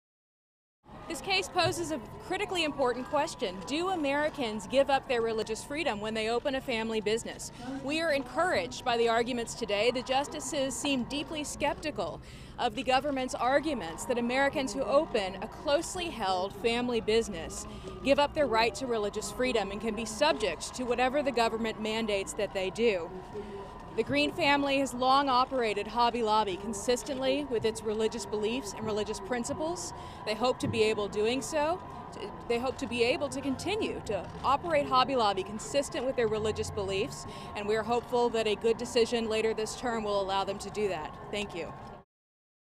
Becket Fund: SCOTUS seemed "deeply skeptical" of government arguments in Sebelius v. Hobby Lobby
The Becket Fund for Religious Liberty's Lori Windham addresses media outside the Supreme Court, following oral arguments in the landmark Sebelius v. Hobby Lobby religious freedom case.